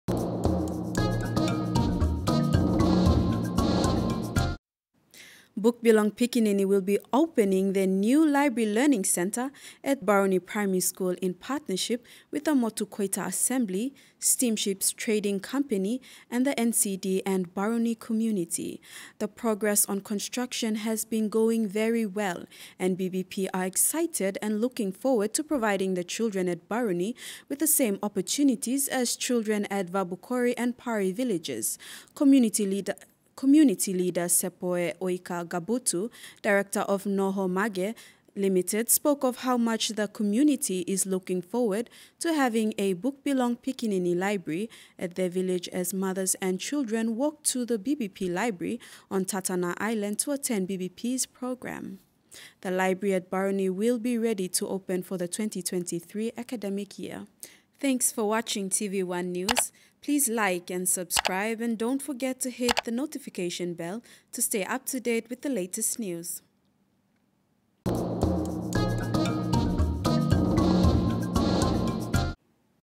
BbP to open Learning Centre 2023
Buk bilong Pikinini will be opening their next new Library Learning Centre at Baruni Primary School in partnership with the Motu Koita Assembly, Steamships Trading Company and the NCD and Baruni community.

The progress on construction has been going very well and BbP are excited to soon the final result and are looking forward to providing the children at Baruni with the same opportunities as the children at Vanukori and Pari villages.